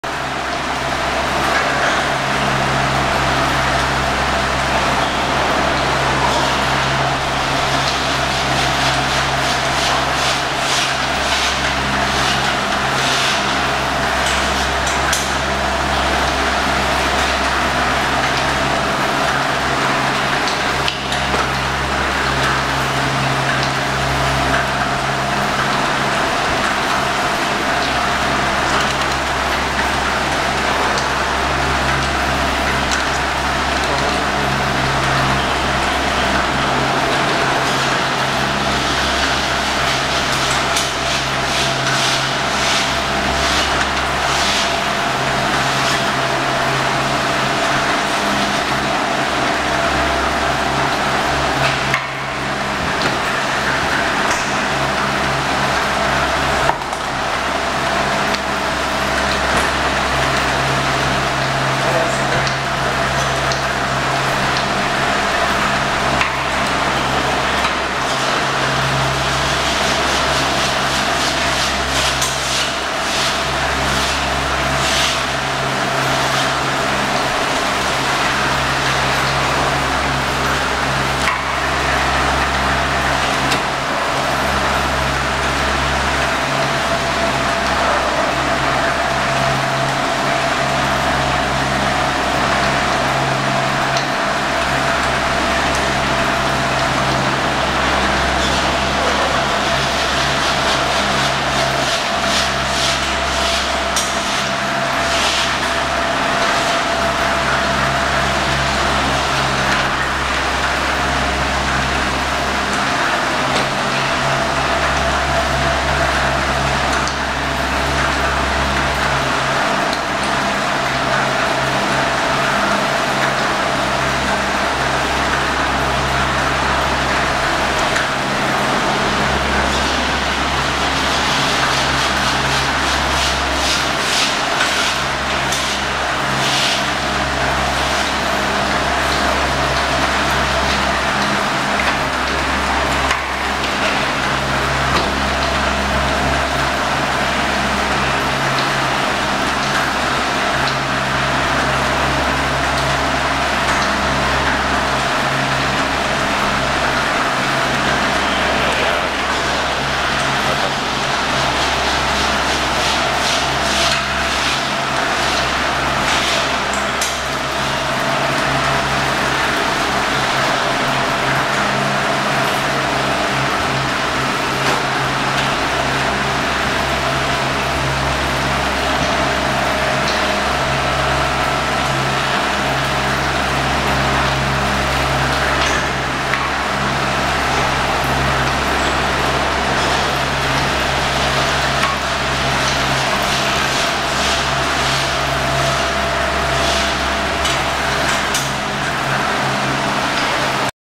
St. Johann in Tirol Garagierung Harschbichl I Talstation
Ein netter Bergbahnmitarbeiter ließ uns bei der Garagierung von Harschbichl zuschauen.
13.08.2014